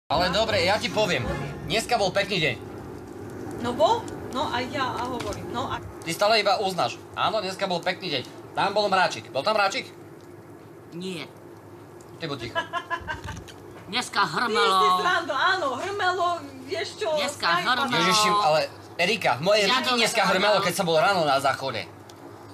Mama ozen ma: v mojej riti hrmelo
Návod ako romanticky zbaliť ženu podľa DJ Mira.